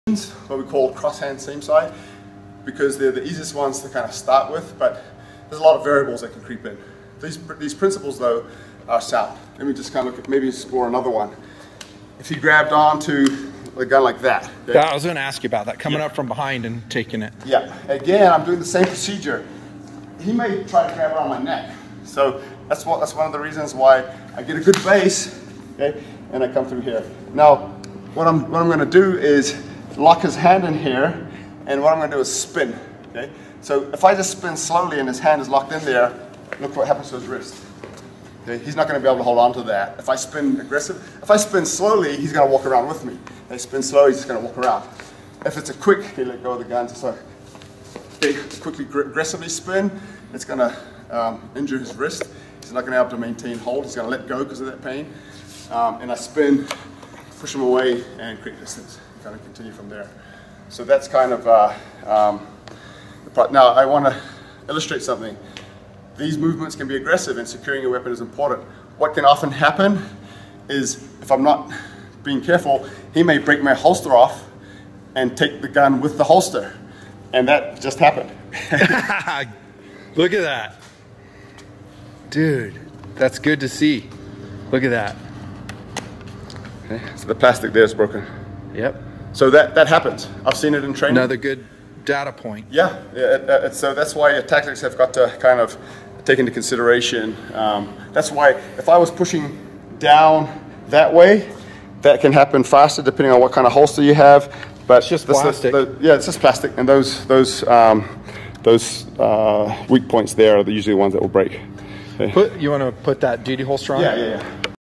“Soft Loops” for Self-Defense
One of the reasons that directly mounted, urethane coated, nylon webbing "soft loops" equipped with Mil-Spec "Pull-the-Dot" hardwear are my preferred method for securely affixing gear to your person.

Also, one of multiple reasons I advocate seriously considering AIWB for those willing to entertain it as an option.

Link to Nutnfancy's original video below.